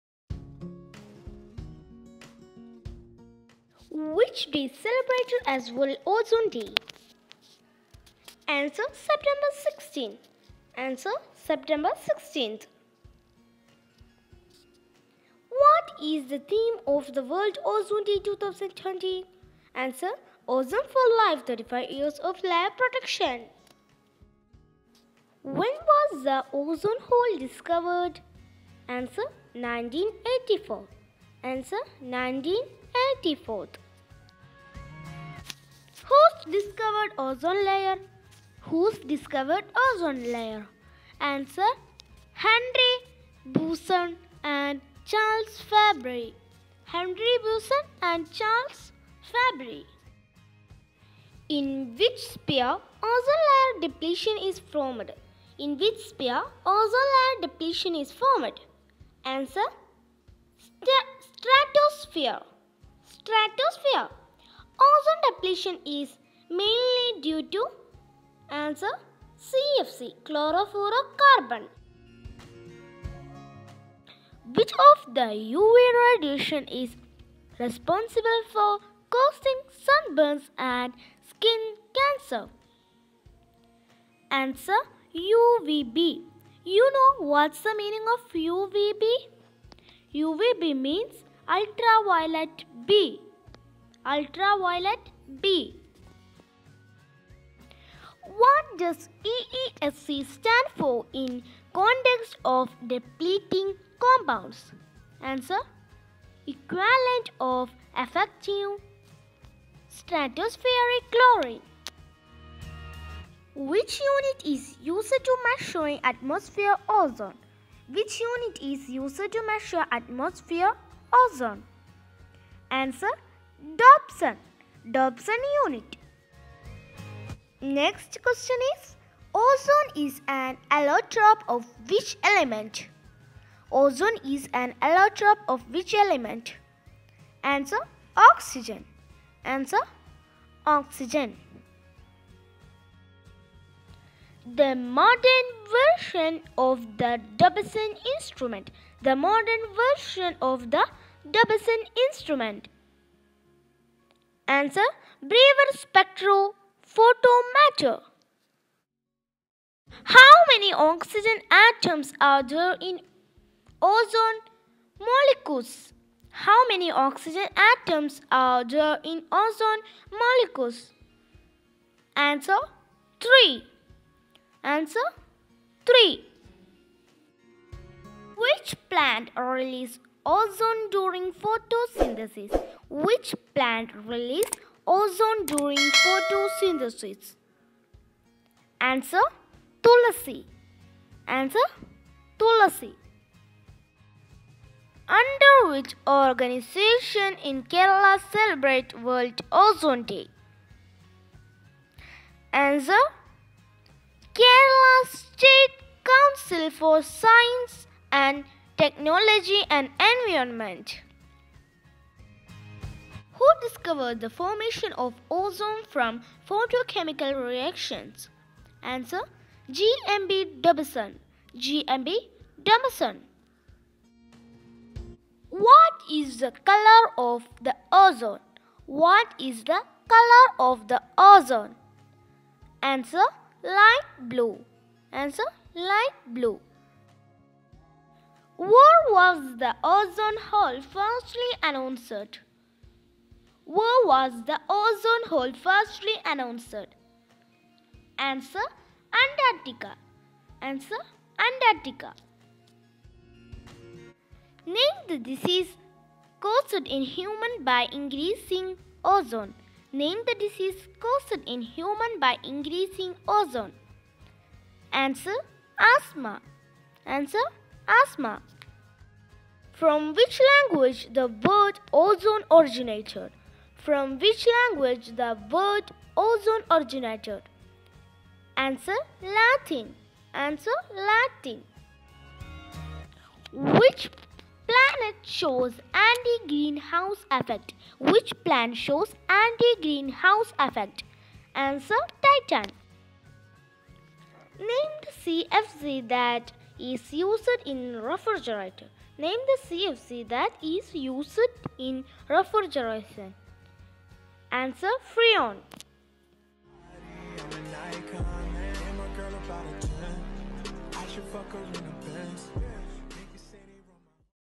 WORLD OZONE DAY QUIZ QUESTIONS AND ANSWERS | Important Question and Answers For World Ozone Day 2020
September 16 Is world Ozone Day

The theme of world ozone Day Is 'OZONE FOR LIFE,35 YEARS OF LAYER PROTECTION '

Please watch my video ,that was helping you to Participate Quiz competition.😍